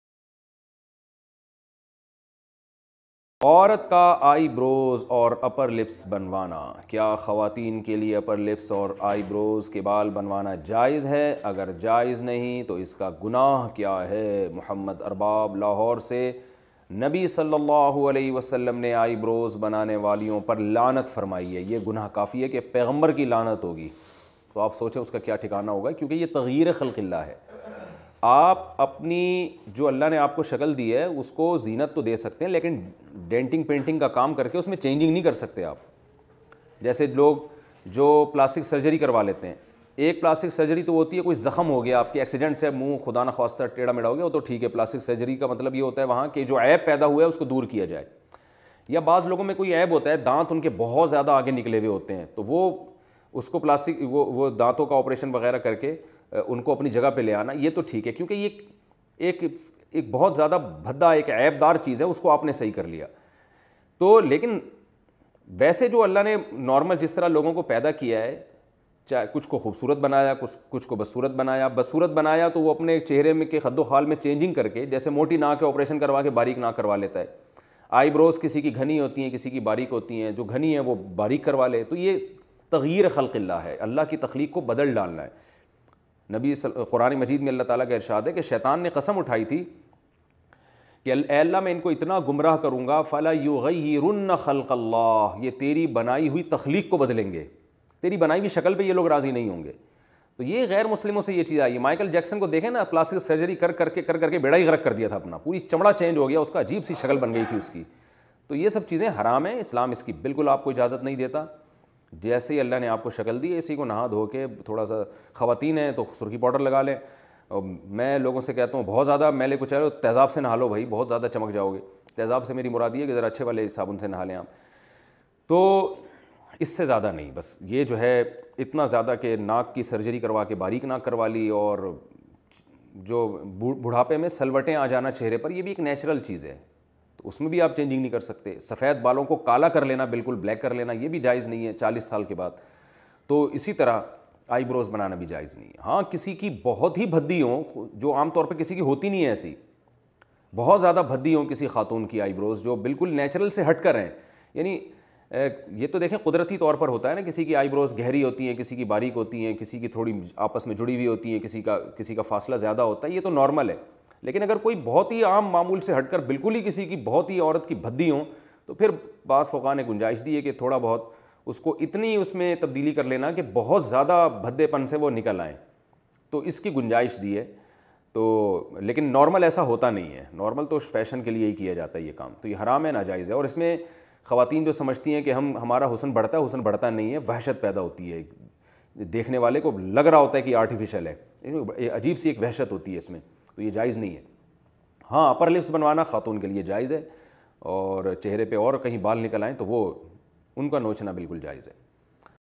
Eyebrows, surgery, braces karwana kaisa hai bayan by mufti tariq masood
braces in islam
eyebrows in islam
surgery in islam